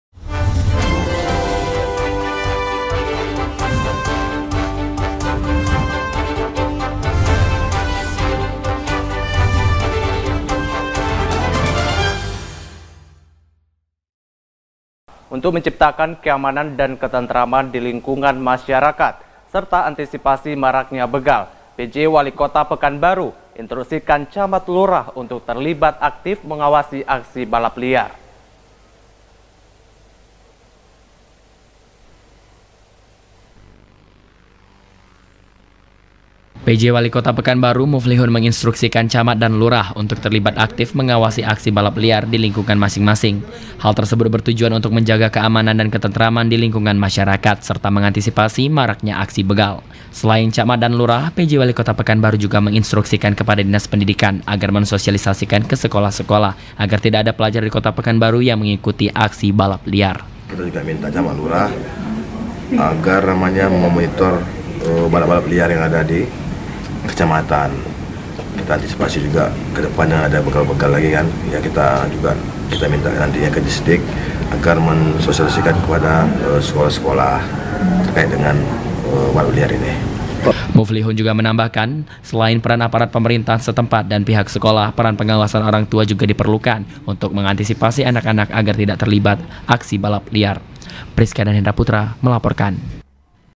PJ WALIKOTA PEKANBARU INSTRUKSIKAN CAMAT LURAH AWASI LINGKUNGAN MASYARAKATNYA
Untuk menciptakan keamanan dan ketentraman di lingkungan masyarakat serta antisipasi maraknya begal Pj Walikota Pekanbaru instruksikan Camat Lurah untuk terlibat aktif mengawasi aksi balap liar.